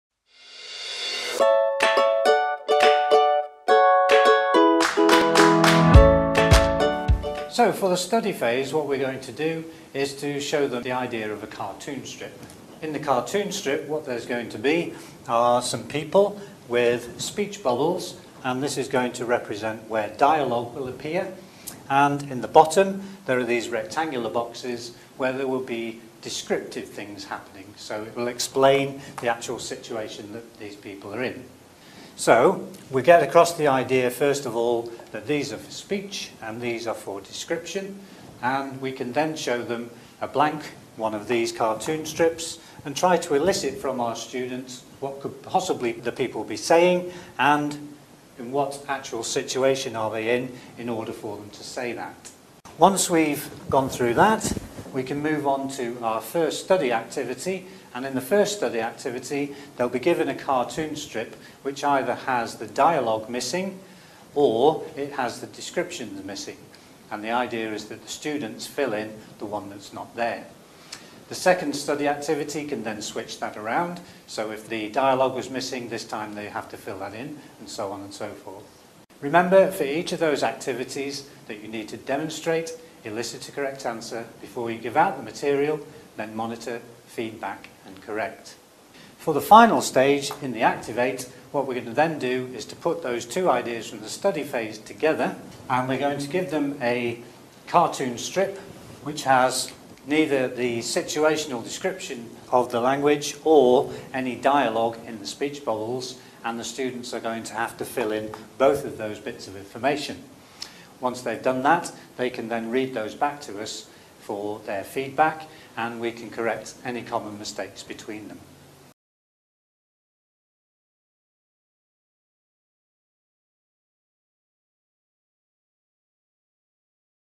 Productive and Receptive Skills in the EFL Classroom - Writing Skills - Study Phase
This video is part of our series on receptive and productive skills in the EFL classroom.
If you haven't watched the previous parts of our receptive and productive skills series, be sure to watch them first:

This video focuses on writing skills in an EFL classroom. For the study phase of a lesson, we could show the students a cartoon strip with speech bubbles and rectangular boxes for descriptive text. The idea is to fill in either the speech bubble or the description.